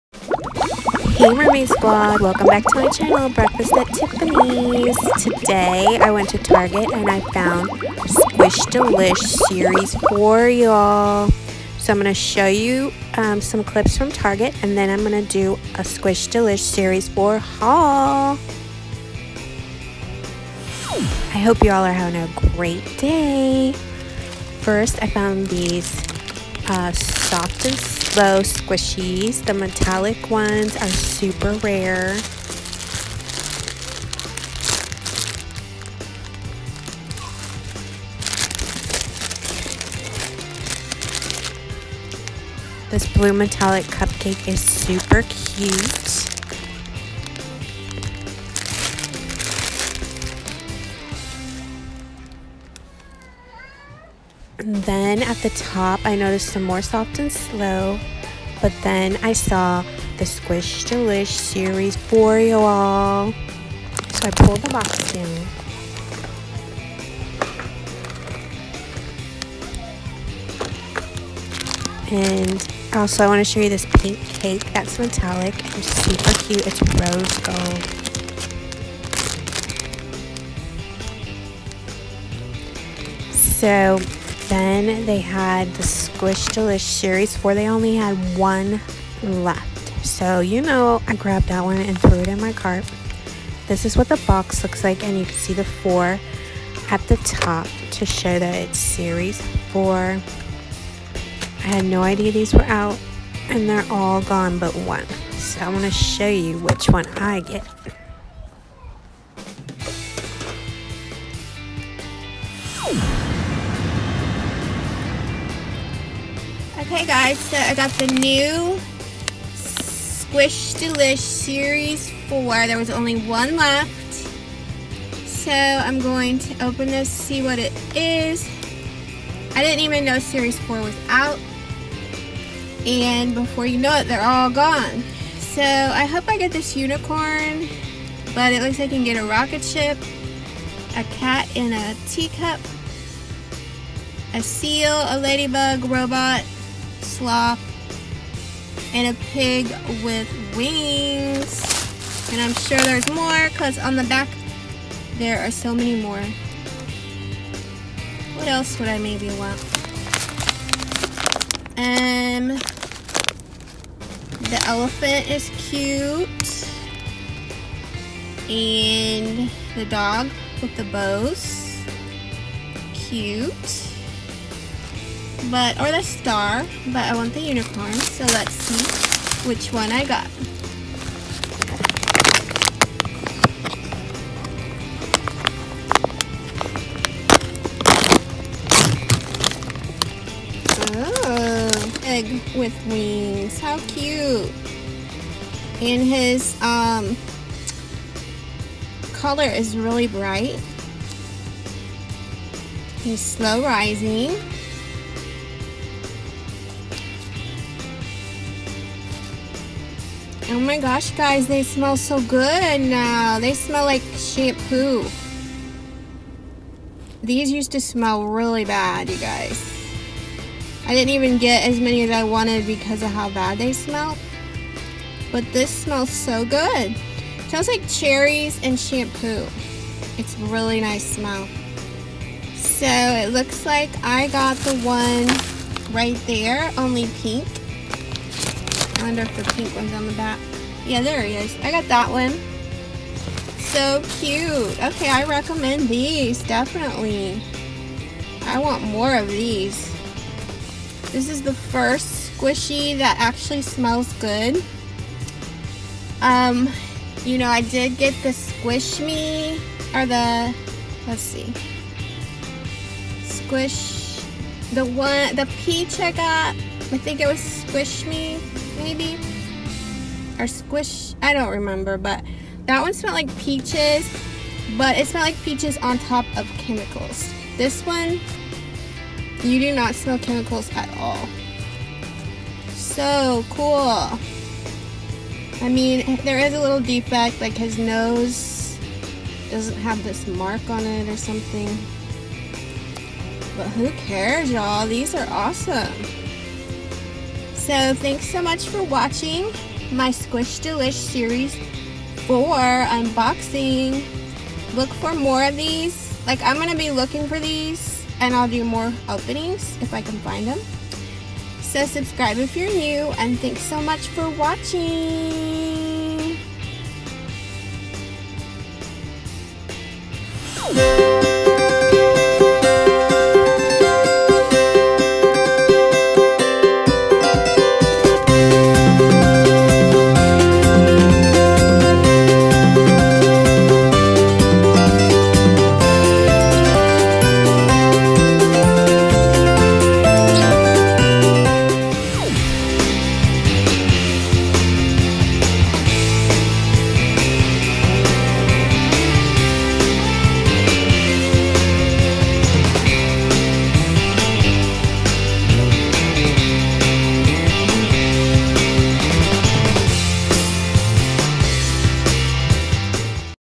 Squish Dee Lish Series 4 at Target | Squish Dee Lish series 4 Haul
Squish dee lish series 4 haul- I found Squish Dee Lish Series 4 SQUISHIES at Target! The squish Dee Lish series 4 box looked empty like everyone had bought the SQUISHIES! But I found one Squish Dee Lish Series 4 blind bag and threw it in my cart! Which one will I get? I really want the Unicorn! Also the Squish Dee Lish series 4 smell so good! They had stepped up their game and the SQUISHIES smell better than any SQUISHIES I've ever smelled!